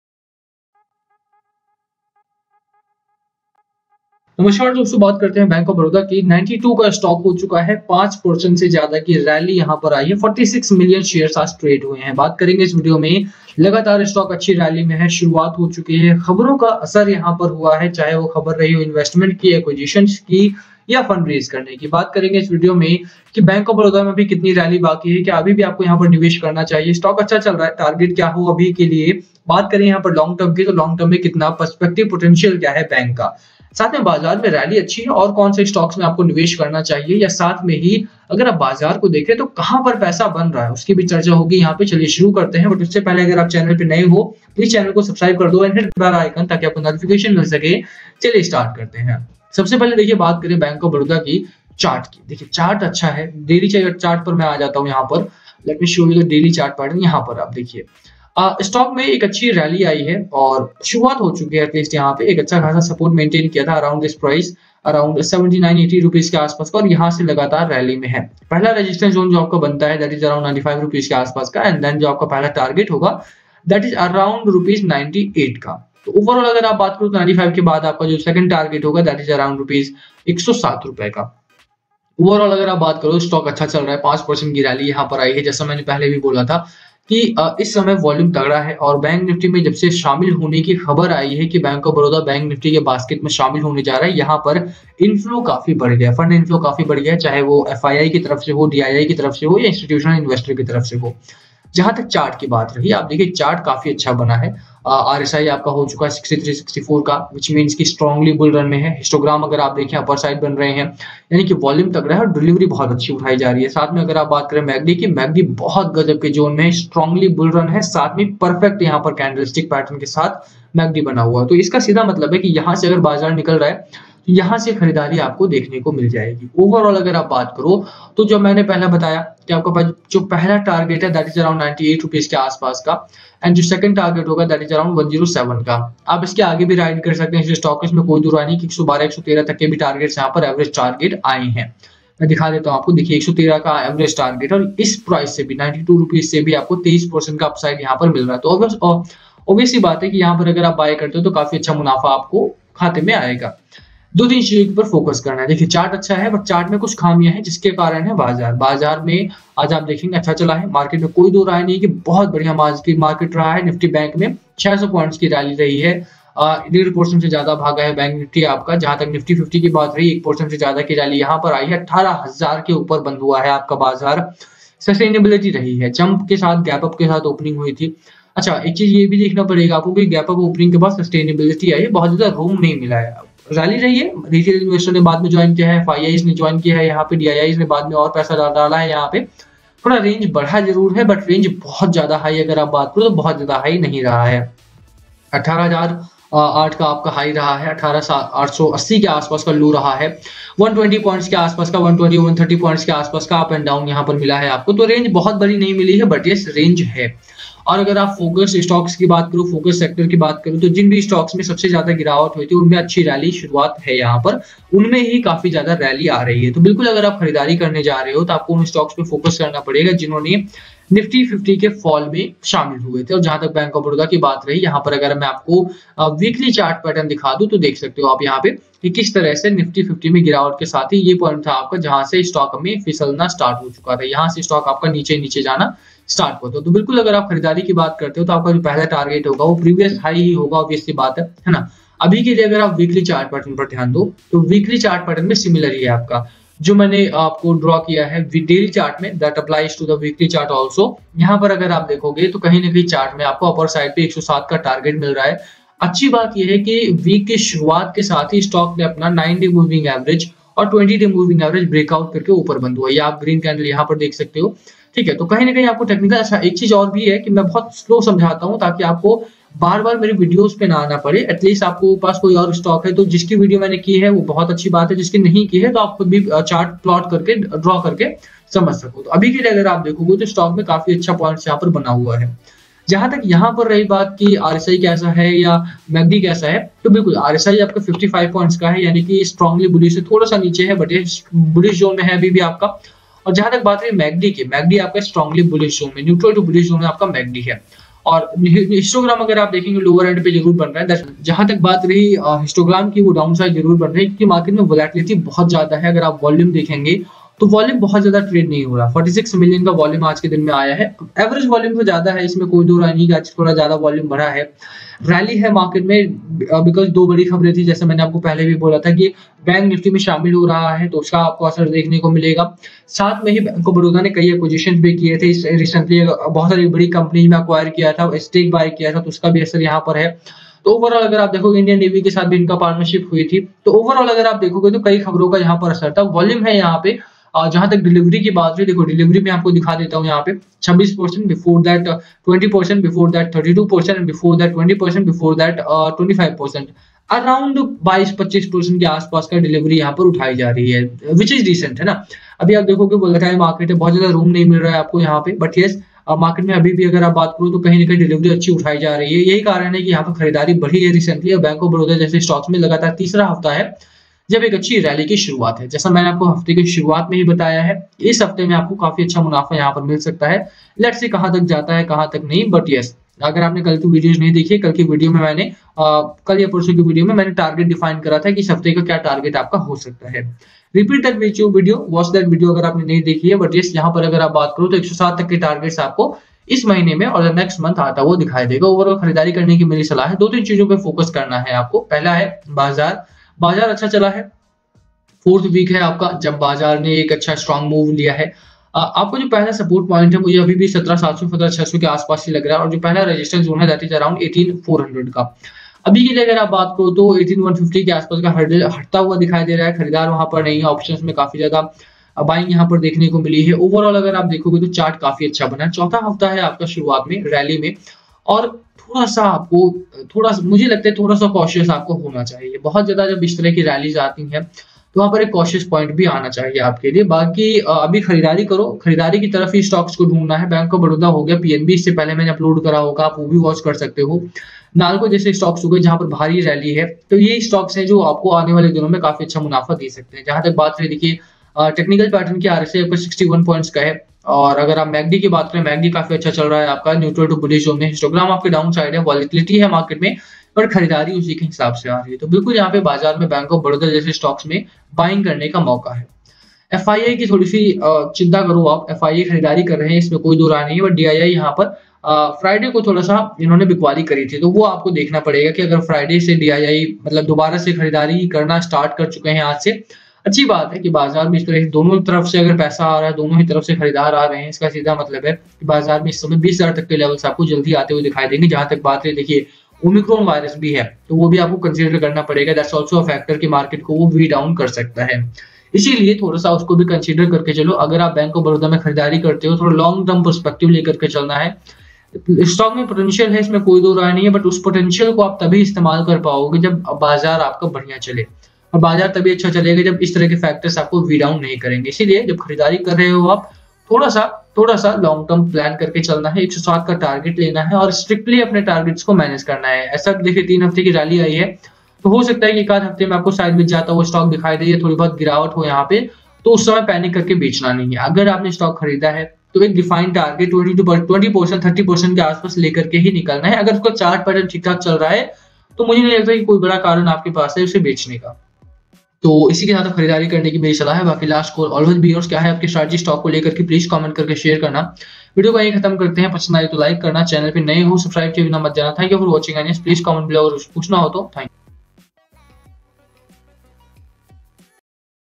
Bank of Baroda Share; करोड़पति या रोडपति, टारगेट आग लगा देगा; bob share price, bob stock news, bob
Bank of Baroda Share Price rose more than 5% today, Should You Invested in Bank of baroda Stock amid rising market.?

bank of baroda ka share kyu gir raha hai,